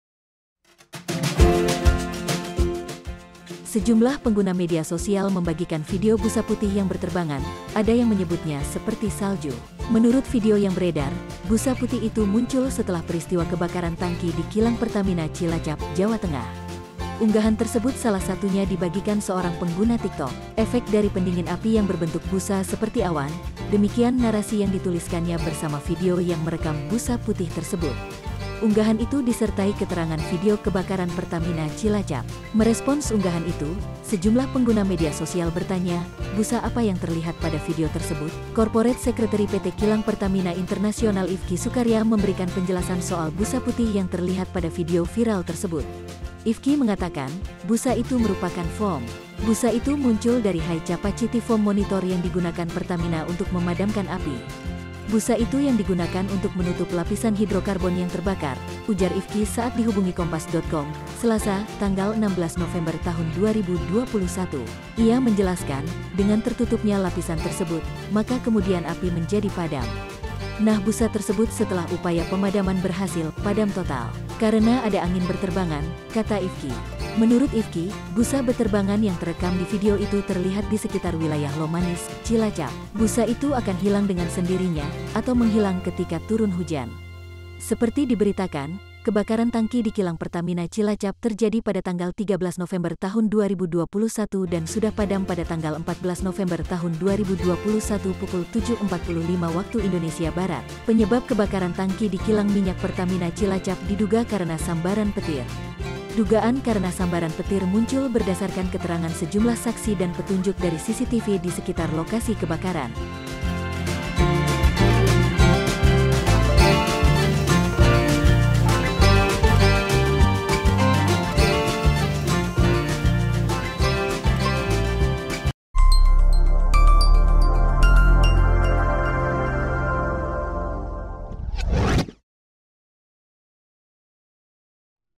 FAKTA Video Busa Putih Beterbangan Bak Salju setelah Kebakaran Tangki di Cilacap
Menurut video yang beredar, busa putih itu muncul setelah peristiwa kebakaran tangki di kilang Pertamina Cilacap, Jawa Tengah. Unggahan tersebut salah satunya dibagikan seorang pengguna TikTok.
“Efek dari pendingin api yang berbentuk busa seperti awan,” demikian narasi yang dituliskannya bersama video yang merekam busa putih tersebut.